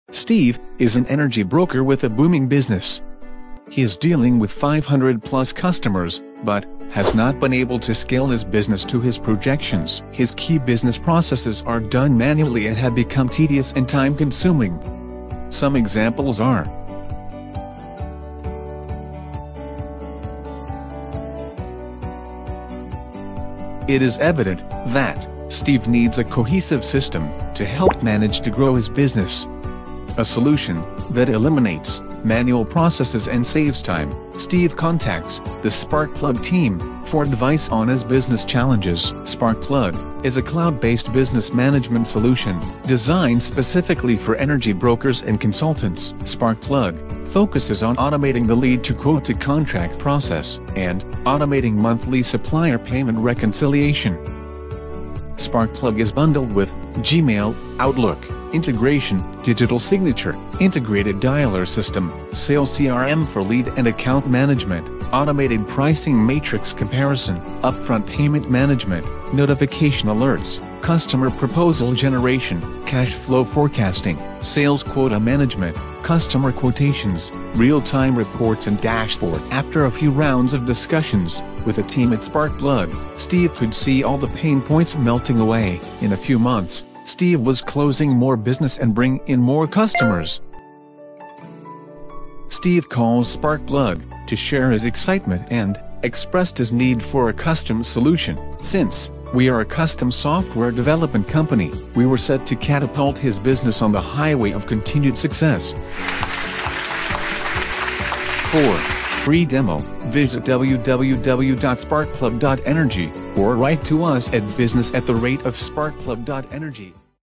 Sparkplug - energy brokers solution by Powermatrix LLC
Sparkplug is an energy broker solution. It is a complete lead to quote to contract management solution along with commission reconciliation.